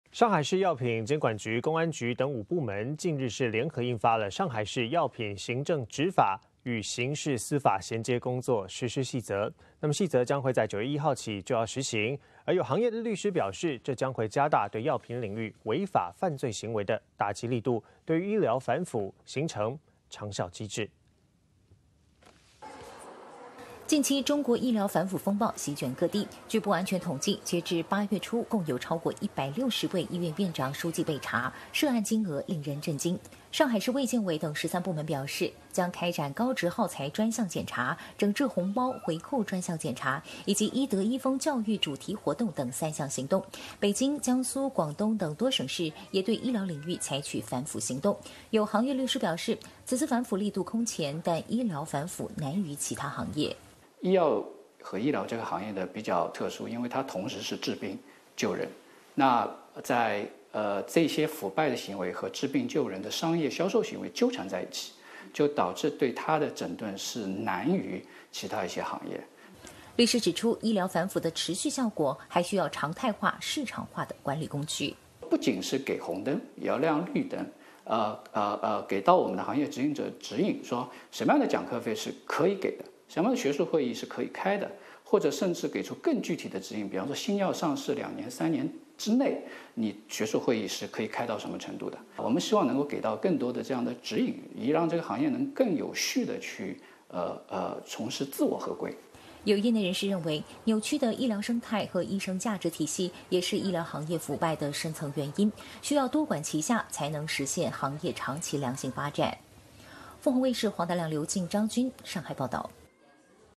【內地醫療反腐】內地醫療反腐力度空前 長效機制更受關注
近期內地醫療反腐風暴席卷各地，據不完全統計，截止8月初，共有超過160位醫院院長、書記被查，涉案金額令人震驚。反腐